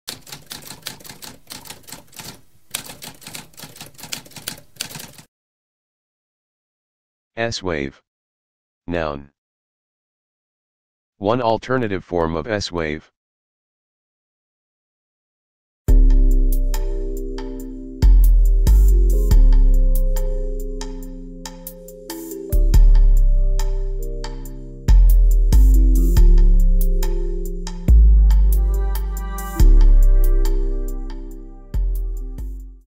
What does S wave mean?
A spoken definition of S wave.

Intro Sound:
Typewriter - Tamskp